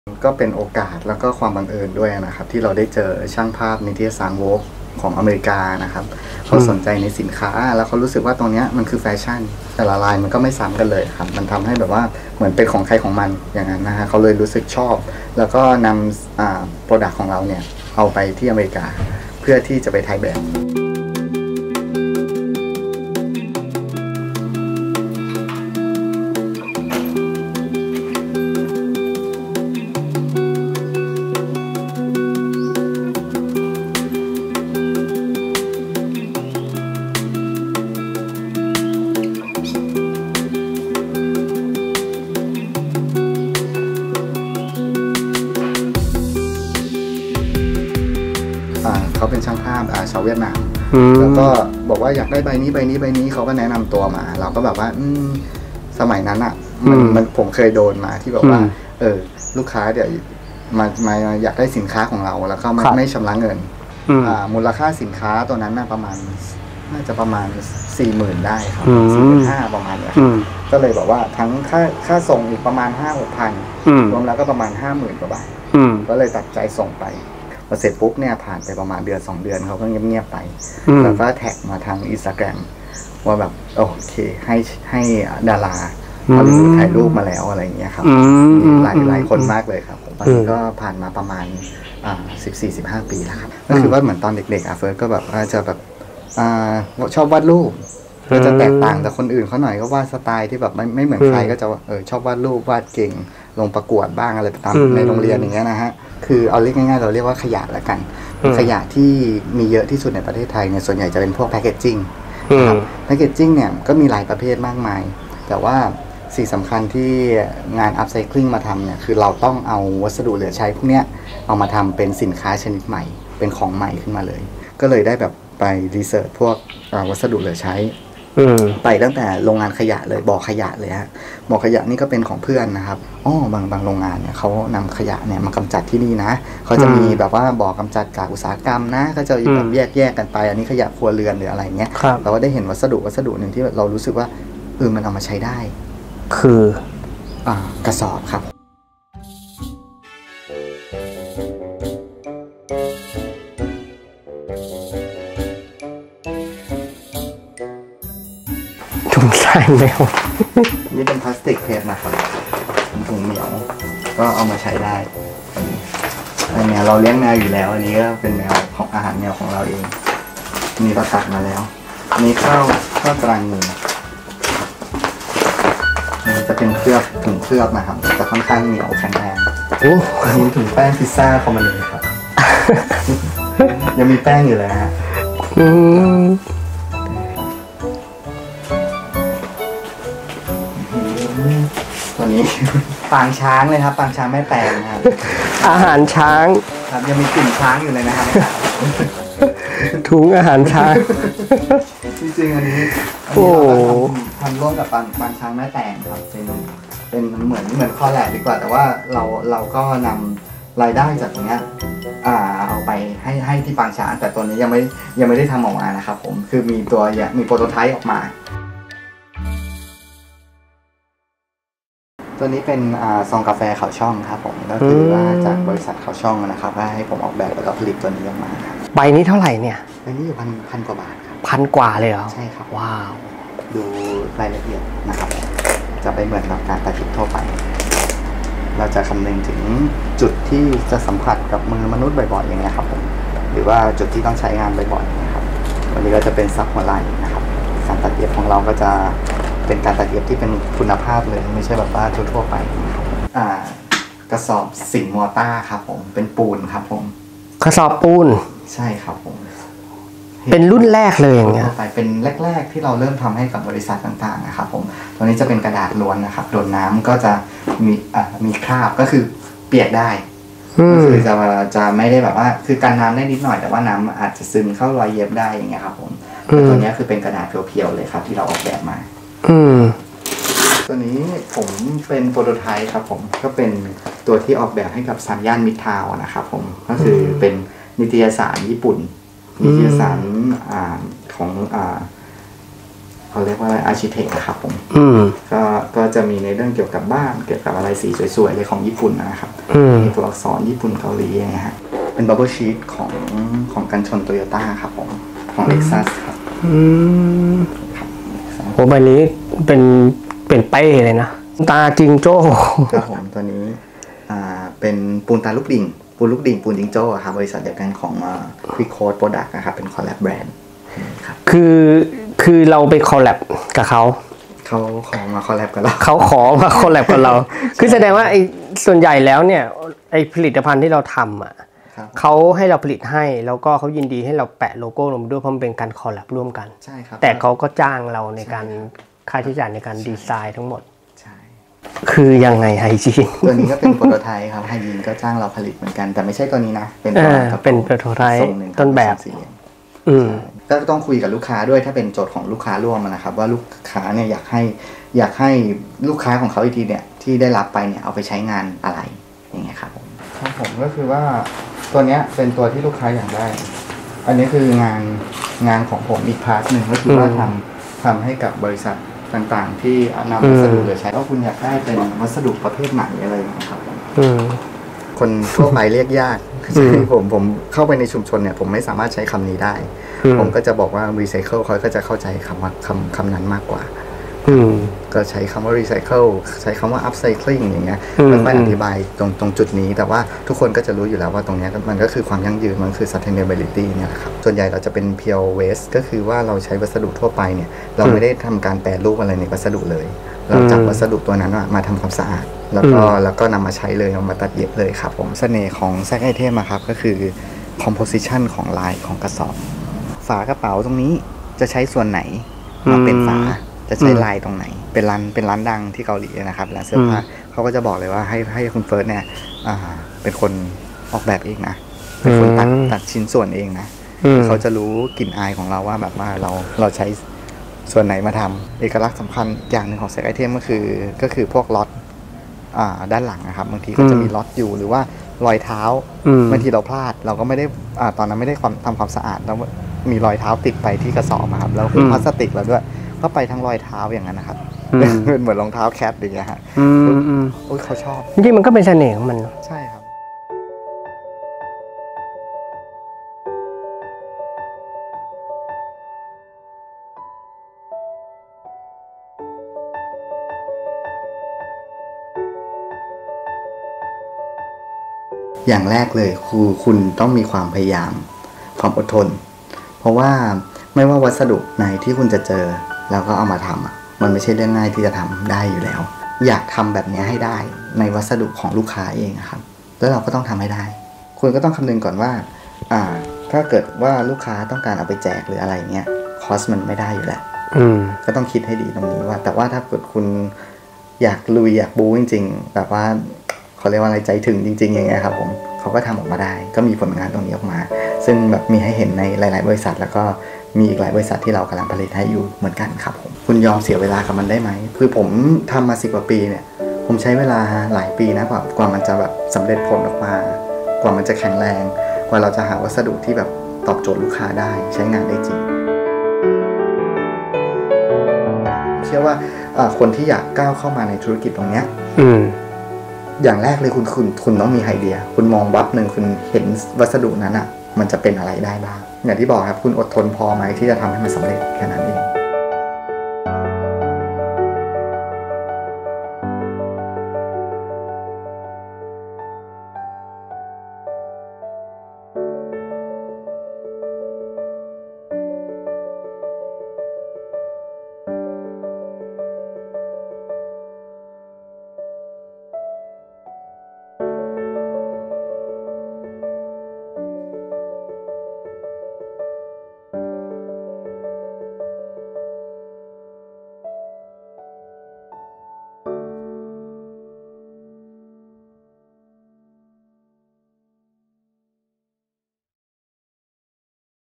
Sack, the money { กระสอบเงิน กระสอบทอง แสน Cool }
กระสอบเก่า เอามาทำเก๋า และสร้างมูลค่าเพิ่ม เปลี่ยนขยะกลายไปเป็นเงิน เป็นทอง แถมดังไกลถึง Hollywood พูดคุยกับ คุณเฟิส ธนารักษ์ วรฤทธานนท์ ผู้ก่อตั้ง Sackitem แฟชั่นกระเป๋าเก๋า ๆ กันครับ

Directed & Filmed by:
ดร.พิชัย ภู่สัมพันธ์
Dr. Pichai Pusumpun

บอกเล่าเรื่องราวดี ๆ แบบมีมู้ด ติดต่อที่: